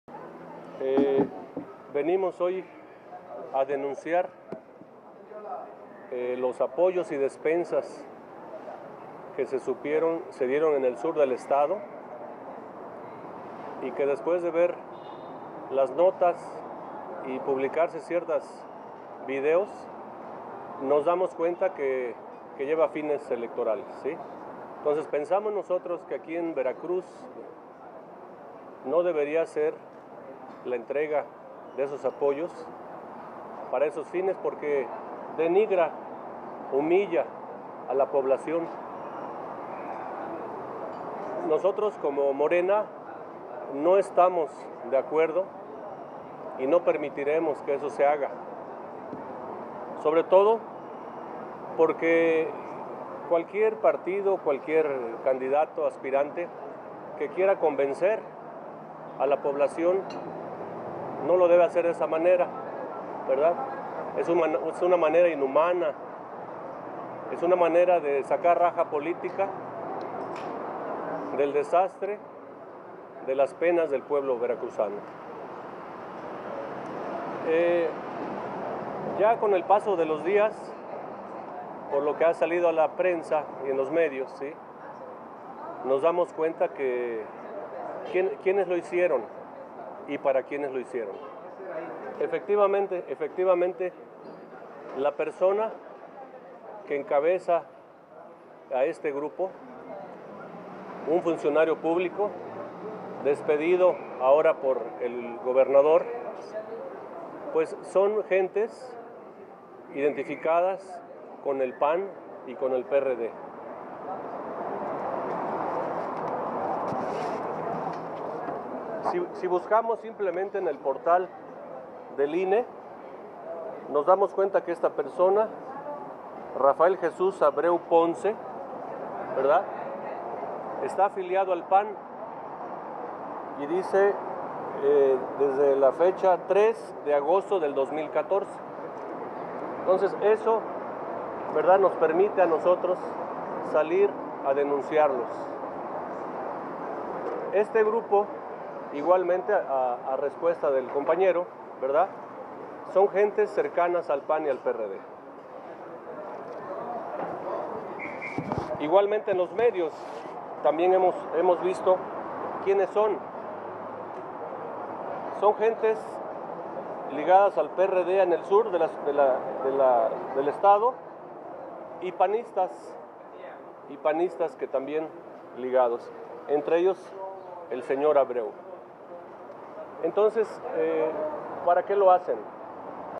Denuncian que el gobierno se aprovecha de la situación de las personas
Xalapa, Veracruz a 13 de Septiembre de 2017.- Los representantes del Movimiento de Regeneración Nacional (Morena) a través de Guillermo Fernandez Sanchez coordinador de la casa de gestión del Diputado Federal Cuitláhuac García Jiménez, el partido Morena realizó la denuncia ante la Fiscalía Especializada para la Atención de Delitos Electorales (Fepade) contra los panistas por el uso del logotipo "Yunete" el pasado sábado 9 de septiembre.

Y ante el Organismo Público Local Electoral (OPLE) Veracruz se hizo la queja para que sea investigado el partido por la supuesta utilización de recursos públicos para la entrega de apoyos y despensa en el municipio de Coatzacoalcos.